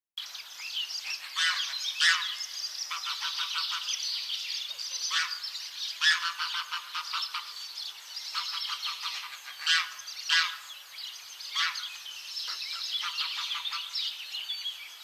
🎴 A FAMILY OF GEESE IS COOLING OFF
Geese in Vovousa Zagorochoria Greece - Χήνες στην Βωβούσα Ιωαννίνων στα Ζαγοροχώρια